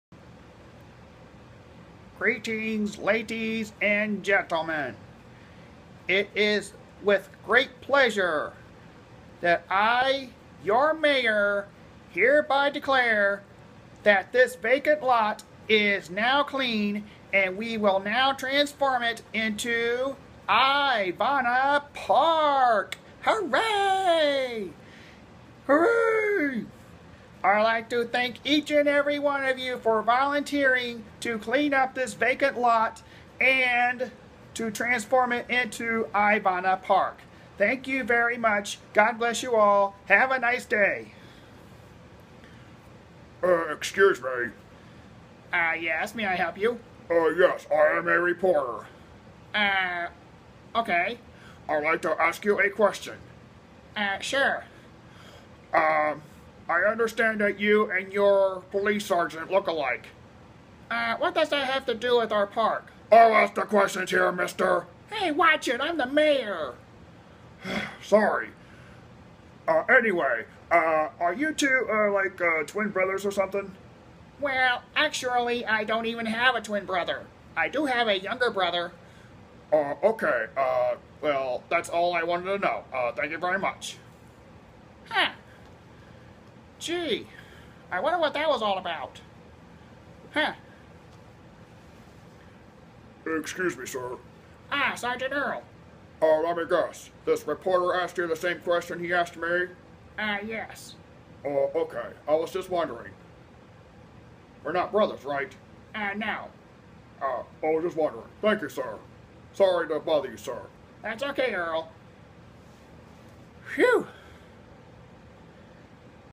Mayor's Announcement & Interview
The mayor makes a speech on opening Ivana Park & gets a visit from a reporter.